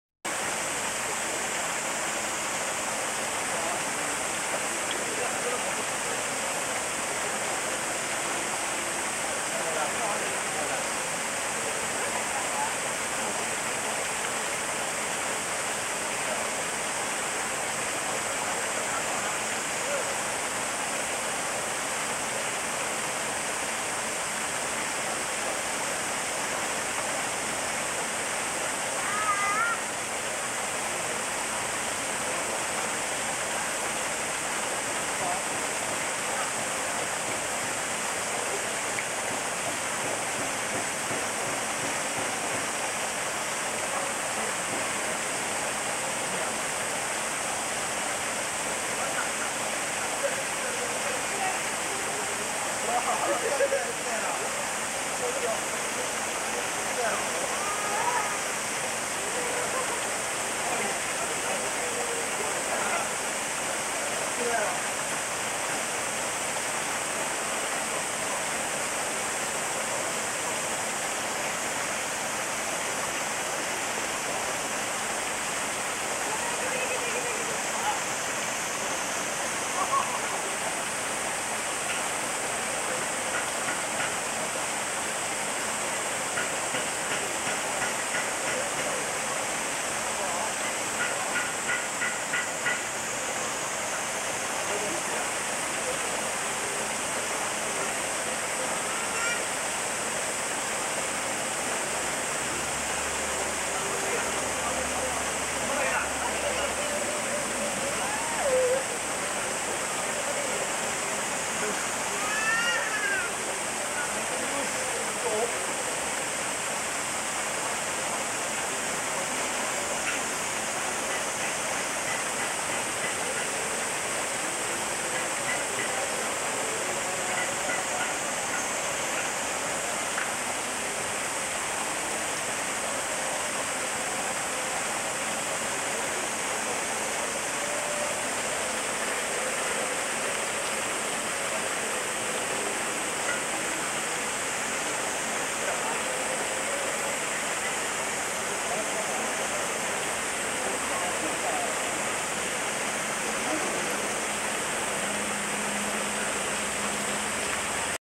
有馬ヘルスセンター マス釣りで釣ったものを夕食
有馬ヘルスセンター1962年開業
2001年には有馬ビューホテル別館日帰り温泉となり
有馬ビューホテルの一部として運営されていた
現在は「太閤の湯」としてリニューアル

当時一泊二日 12,000円とリーズナブル
館内施設は充実、マス釣りで釣ったものを
一匹1500円で刺身用に裁いて夕食に頂きました
活気のあった昭和の名残ですね

平成元年7.22に撮影した

有馬ヘルスセンター
神戸市北区有馬町池の尻292-2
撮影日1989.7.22